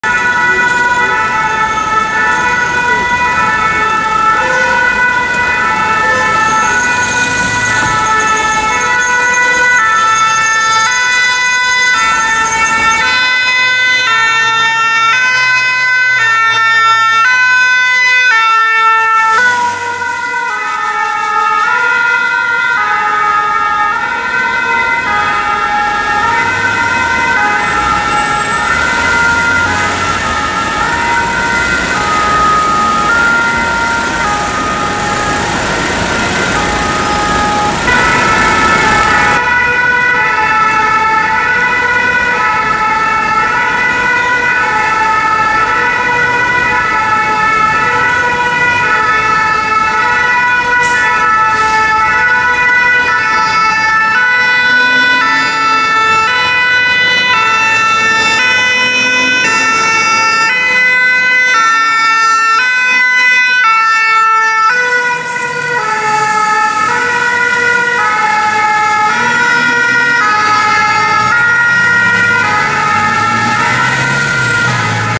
VSAV + EPA SDIS 69 CT Lyon-Rochat
en urgence sur le Cours Gambetta
an emergency ambulance and a ladder truck of the Lyon Fire Department responding

by l'ambulance verte
Lyon, Rhône, Rhône-Alpes, France
11/2011